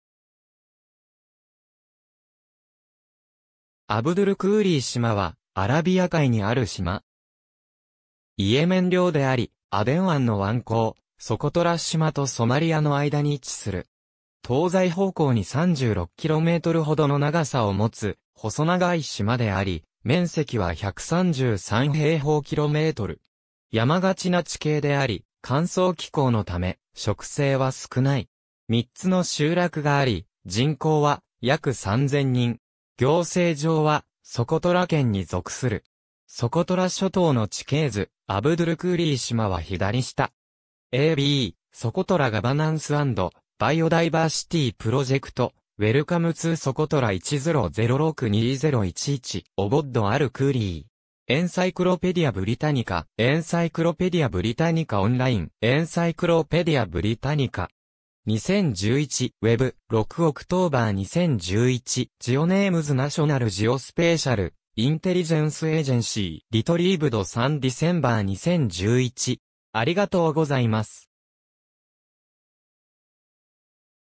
アブドゥルクーリー島
アブドゥルクーリー島（アブドゥルクーリーとう、アラビア語: وعبد الكوري‎）は、アラビア海にある島。
イエメン領であり、アデン湾の湾口、ソコトラ島とソマリアの間に位置する。
東西方向に36kmほどの長さを持つ細長い島であり、面積は133km2。
山がちな地形であり、乾燥気候のため、植生は少ない。
3つの集落があり、人口は約3千人。
行政上はソコトラ県に属する。
ソコトラ諸島の地形図、アブドゥルクーリー島は左下。
^ a b Socotra Governance & Biodiversity Project - "Welcome to Socotra" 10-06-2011. ^ "ʿAbd al-Kūrī." Encyclopædia Britannica. Encyclopædia Britannica Online. Encyclopædia Britannica,
2011. Web. 6 October 2011. ^ GeoNames National Geospatial-Intelligence Agency. Retrieved 3 December 2011.